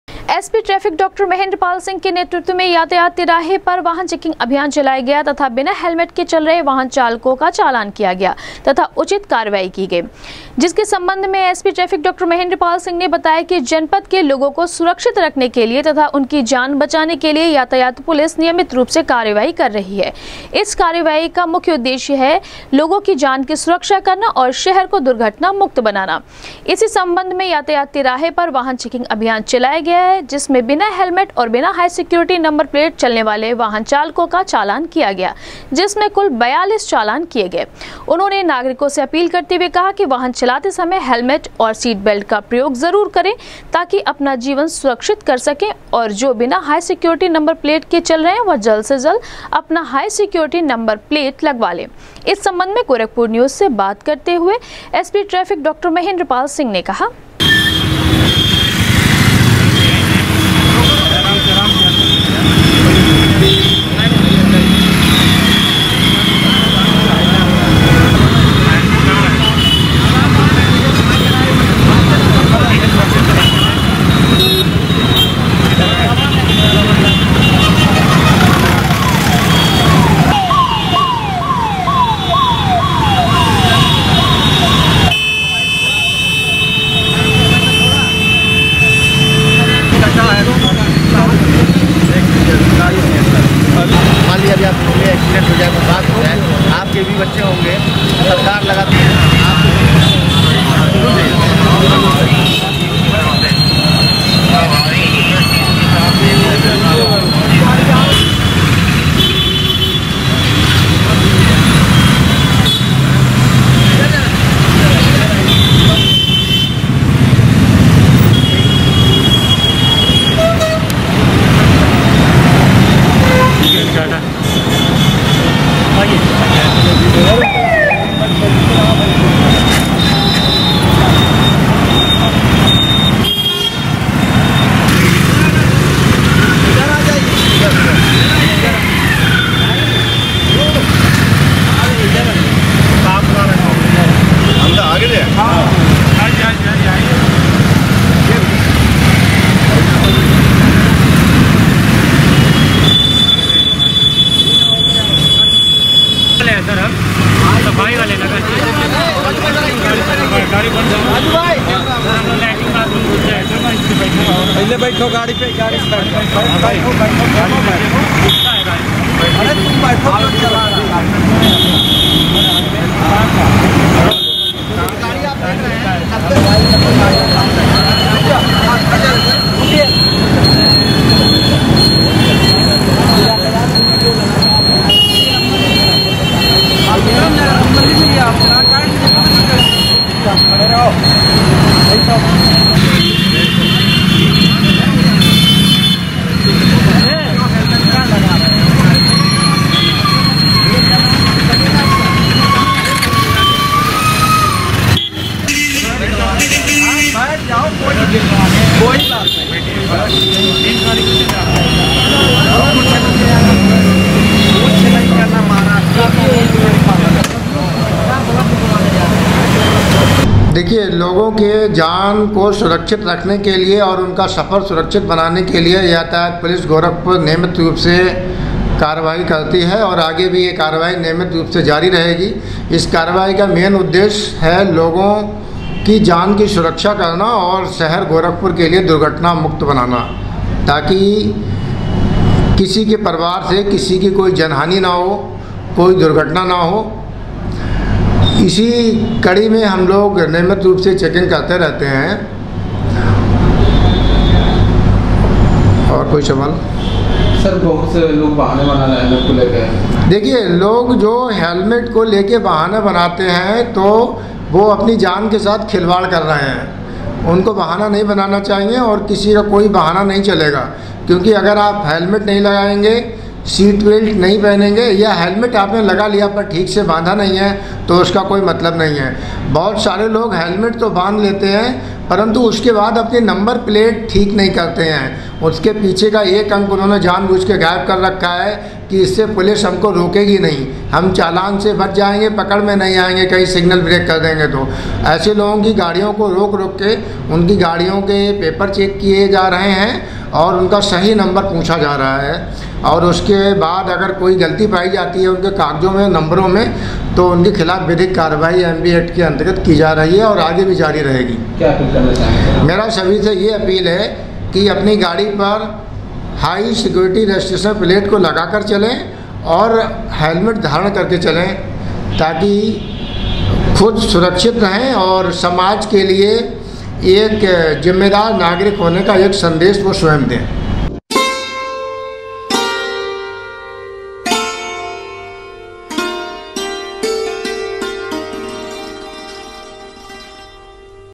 एसपी ट्रैफिक के नेतृत्व में चलाया गया वाहन चेकिंग अभियान, बिना हेलमेट के चल रहे वाहन चालकों का
एसपी ट्रैफिक के नेतृत्व में चलाया गया वाहन चेकिंग अभियान, बिना हेलमेट के चल रहे वाहन चालकों का किया गया चालान,एसपी ट्रैफिक डॉ महेंद्र पाल सिंह ने कहा, लोगों की जान की सुरक्षा और शहर को दुर्घटना मुक्त बनाना है ट्रैफिक पुलिस का मुख्य उद्देश्य